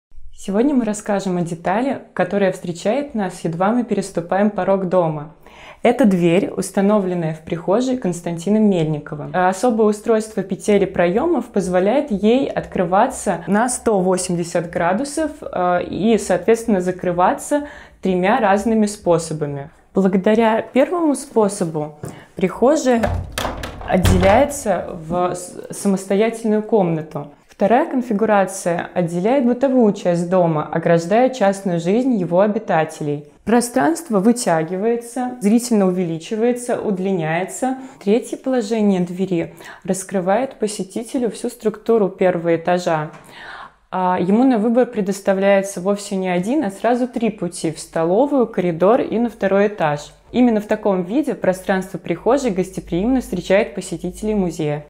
Дверь в доме Мельникова
Научный сотрудник музея Мельниковых Патриция Семенюк расскажет о детали, встречающей каждого гостя дома Мельникова - двери в прихожей.